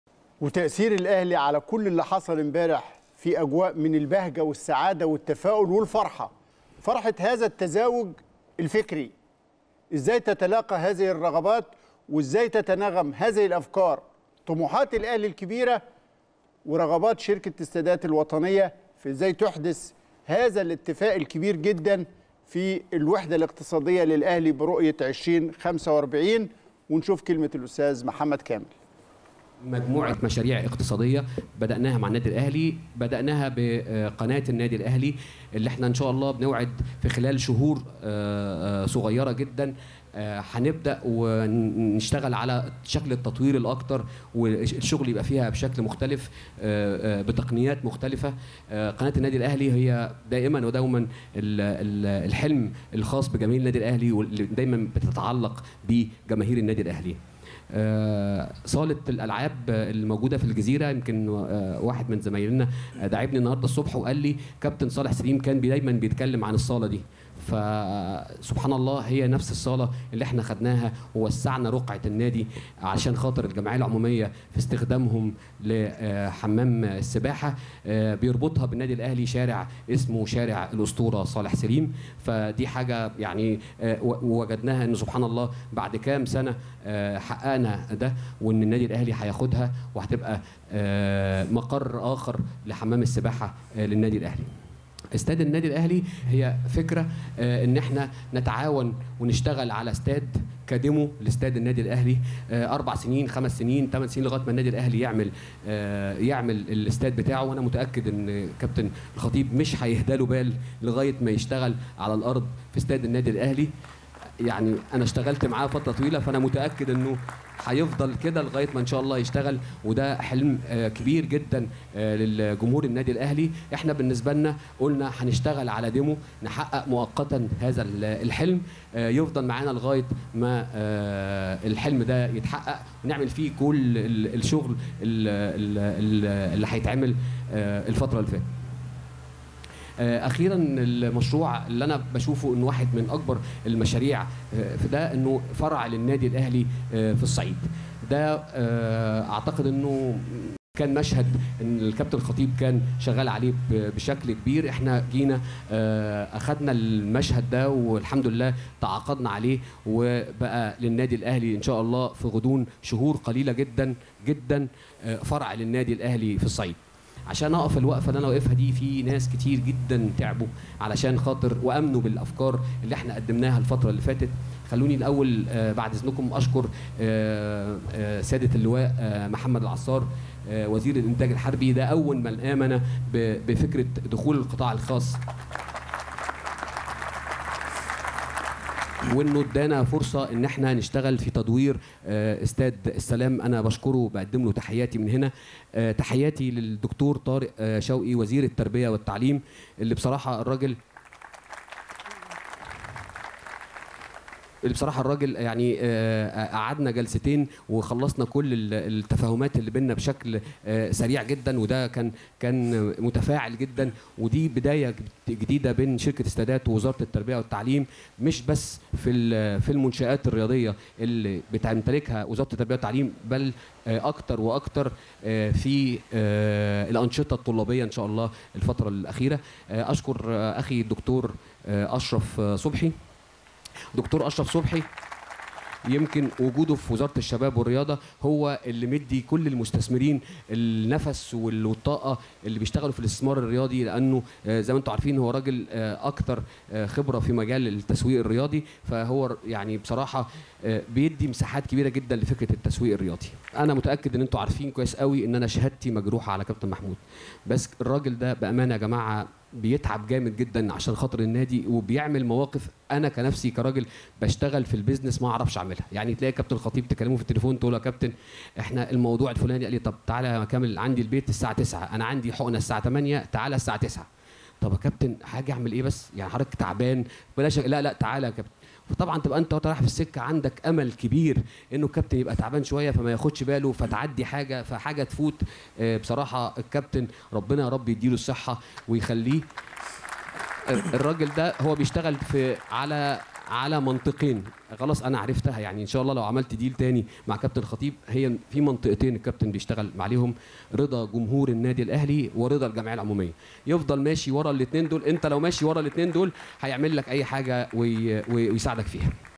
ملك وكتابة وتأثير النادي الأهلي بعد توقيع برتوكول التعاون مع شركة استادات
ملك وكتابة وتأثير النادي الأهلي بعد توقيع برتوكول التعاون مع شركة استادات.. ويستعرض كلمة الاستاذ "محمد كامل".

التردد: 12341 أفقي.
القمر: نايل سات.
معامل الخطأ: 5/6.
معدل الترميز: 27500